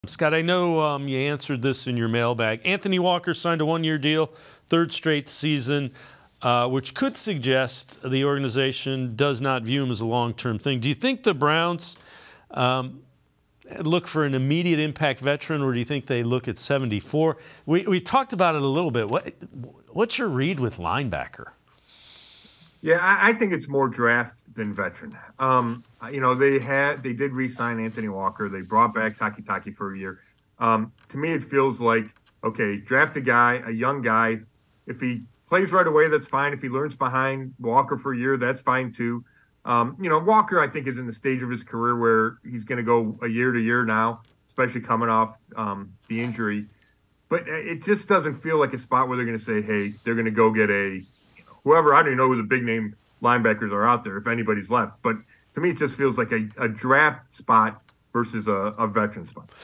How Will the Browns Handle Anthony Walker & the Linebackers Moving Forward? - Sports4CLE, 4/24/23
Dave Bacon and Scott Petrak discuss the Browns at linebacker moving forward.